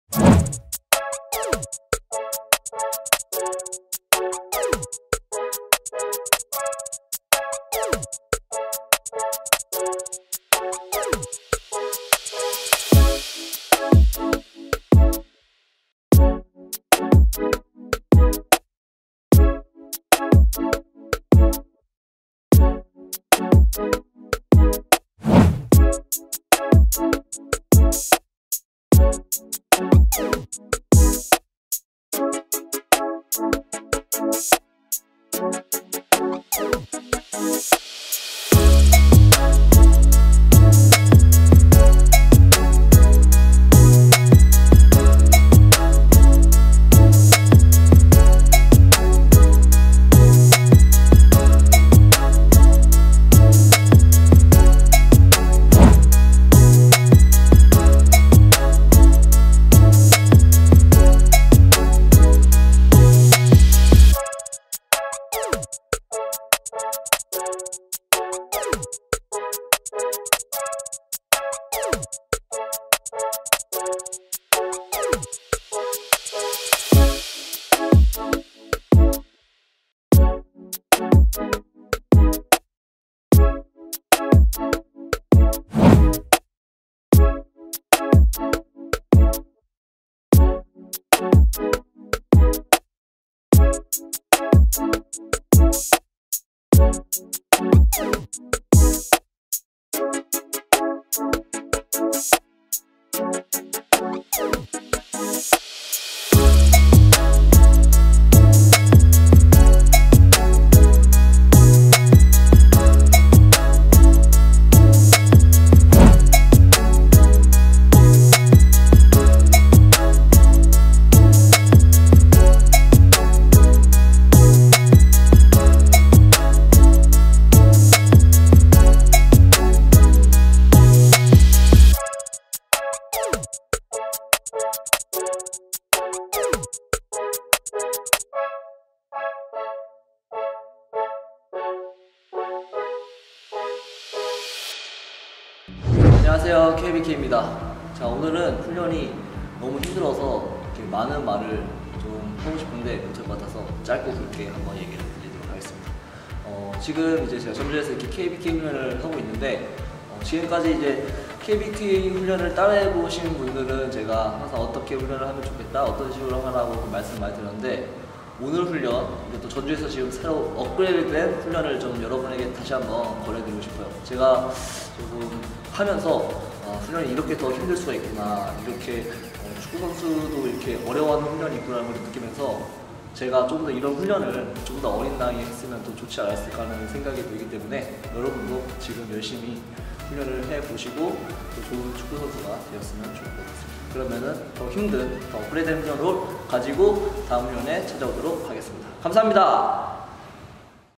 [훈련일지]축구선수도 어려워하는 운동! (Feat. 내가 한 살만 더 어렸어도...)
안녕하세요. KBK 김보경입니다.

오늘은 훈련이 너무 힘들어서 많은 말을 하고싶은데 못할것 같네요 ^^;;

제가 훈련을 하면서 이렇게 축구선수도 어려워하는 훈련이 있구나 하는 생각이 들었어요. 오히려 이런 훈련을 조금 더 어릴때부터 시작했으면 어떘을까 하는 생각도 들었습니다.

1. Gymball inverted row / 10reps, 10reps x 4set
- 짐볼에 고정한 발이 흔들리지 않게 신체 정렬 유지

2. Ultinate Floor press / 16kg 10reps, 10reps x 4set
- 견갑의 고정과 체스트업 유지
- 코어에 긴장도를 유지하며 실시

3. Side Plank row(band) / 15reps, 15reps x 4set
- 머리부터 발끝까지 일직선이 되도록 정률이 무너지지 않게 유지

4. Sprinter clean(band resistace) / 15reps x 4set
- 골반, 무릎, 발목관절을 살짝 굽협다가 동시에 펴며 치고 나간다
- 이때 반동을 이용해 덤벨을 슈러그하듯이 들어준다.
- 하체는 점프하듯이 수직방향으로 힘을 전달

5. Bulgarian Split squat Trunk rot(bosu) / Kettle bell 8kg 10reps x 4set
- 보수에 올린 발의 뒤꿈치가 뜨지 않게 주의
- 발바닥에 힘을 주어 실시
- 허리가 만곡되지 않게 척추 정렬 유지

운동을 하시면서 궁금하신 부분이 생기면 댓글로 남겨주세요. 여러분들이 보내주신 궁금증을 모아 답변해 드리겠습니다. :)